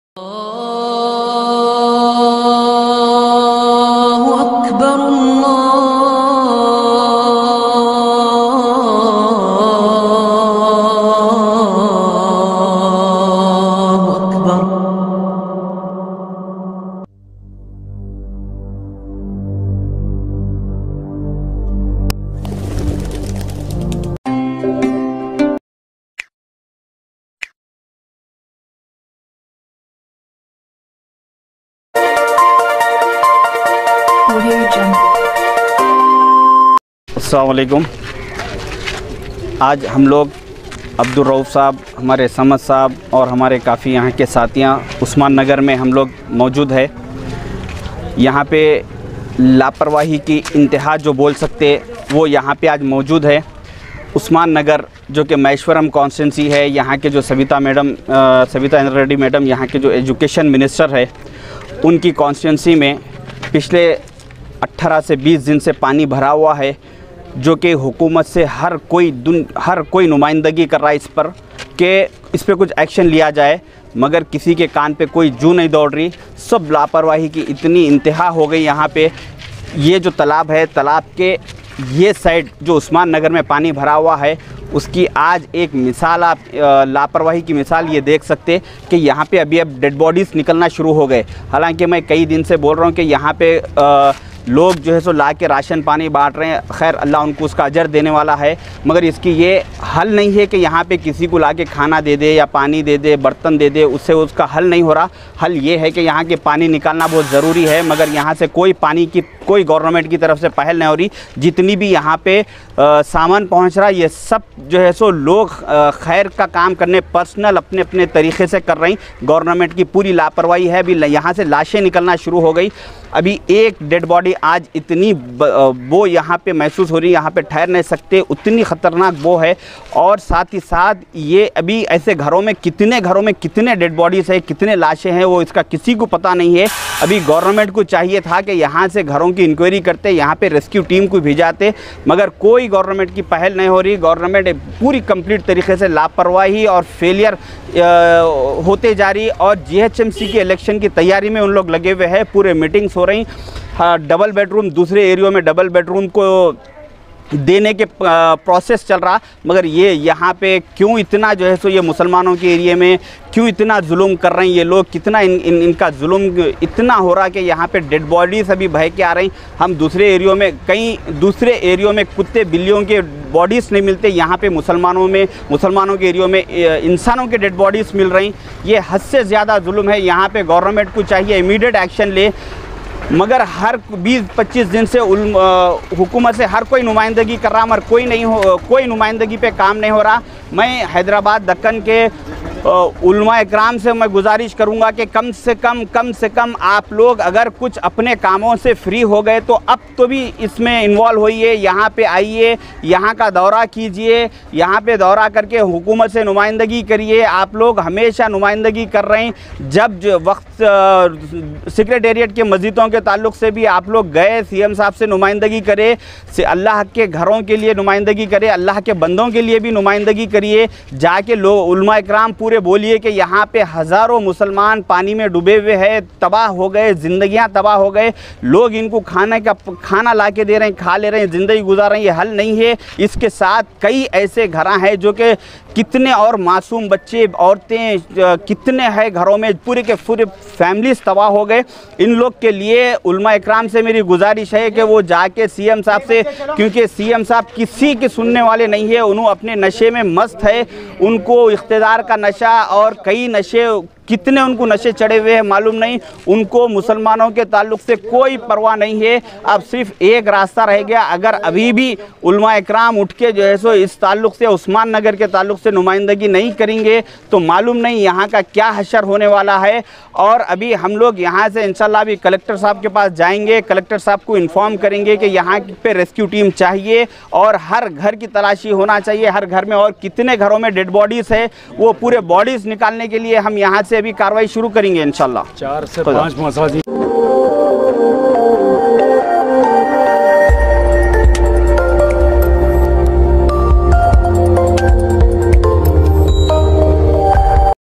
UNKNOWN "DEAD BODY FOUND IN HYDERABAD "OSMAN NAGAR" SHAHEEN NAGAR, HYD| KANJAAL ME PHASI HUI "LAASH"
UNKNOWN "DEAD BODY FOUND IN HYDERABAD "OSMAN NAGAR" SHAHEEN NAGAR, HYD| KANJAAL ME PHASI HUI "LAASH" JALPALLY MUNICIPAL